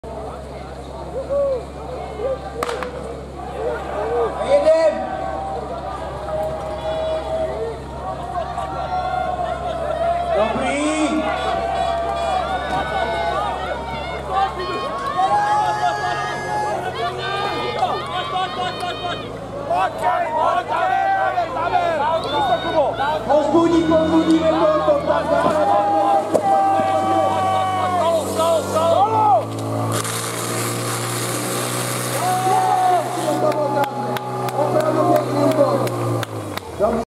SDH Metylovice: Hájov nad 35 let B na noční soutěži v Hukovicích 2018
Hukovice, 5.5.2018, V. ročník noční soutěže O pohár obce Bartošovice